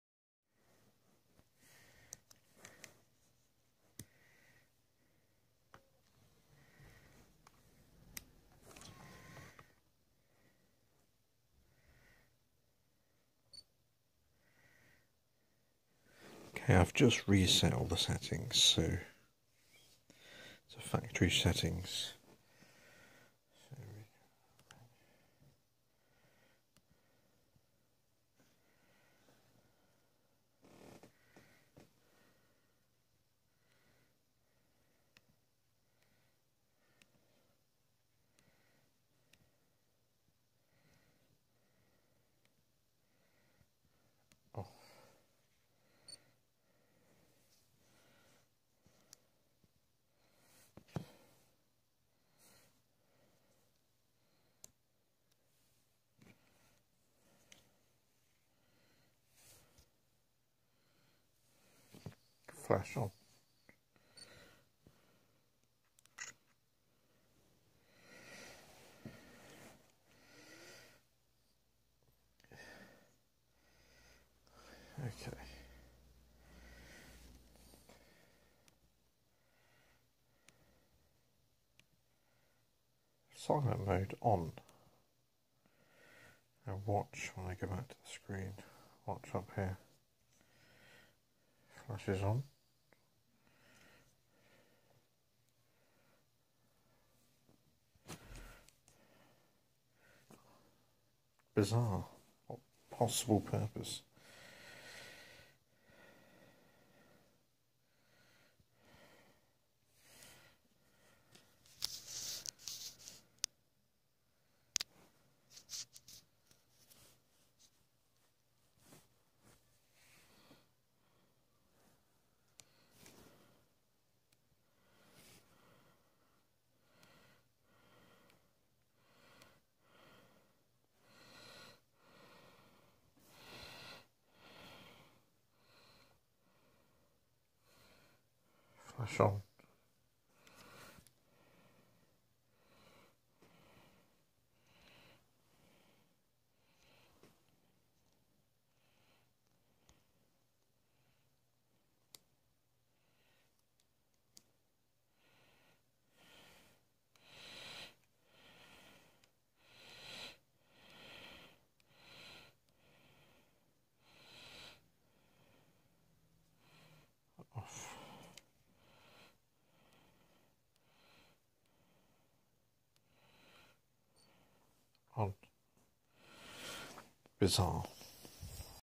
Lx100 flash off when in silent mode
For the some bizzar reason, the flash powers off when camera in silent mode (so beep on focus ect turned off) how bizzar?